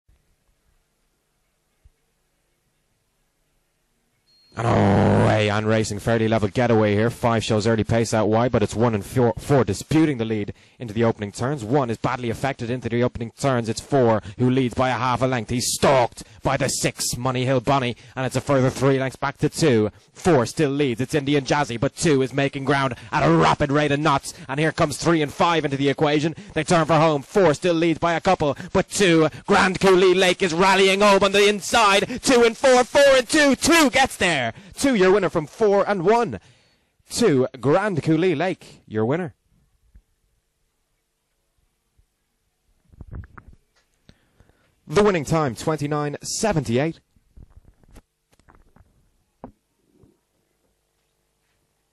IGB - The Try a Lucky Dip A4 18/08/2019 Race 5 - Mullingar
Race: The Try a Lucky Dip A4
Track Name: Mullingar
Date: 18/08/2019
Hour: 16:37

Results

1st    2     GRANDCOULEE LAKE    5/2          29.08
2nd    4     INDIAN JAZZY        13/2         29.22
3rd    1     FAHY RUBY           3/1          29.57
4th    6     MONEYHILL BONNIE    9/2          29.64
5th    5     BECTIVE ABBIGAIL    7/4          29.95
6th    3     GROVE THUNDER       6/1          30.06

|MGR_2019_08_18_05